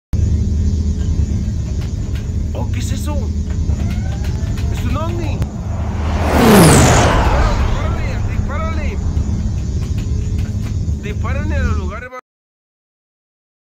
FXGURO La aplicación que esta de moda Descárgala gratis Play Store
Hola amigos de YouTube que tal Aquí les dejo esta gran app muy buena esta gratis en Play Store y es lo que anda ahorita en edición en tu Android